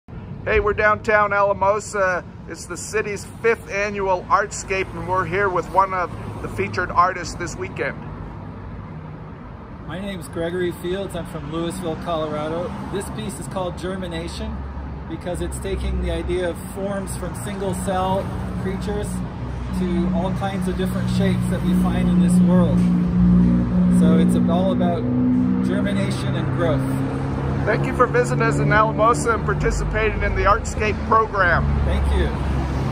Alamosa ArtScape 2021 – artist Gregory Fields
Artist Gregory Fields talks about his piece “Germination,” displayed on Main Street in front of Alamosa Home.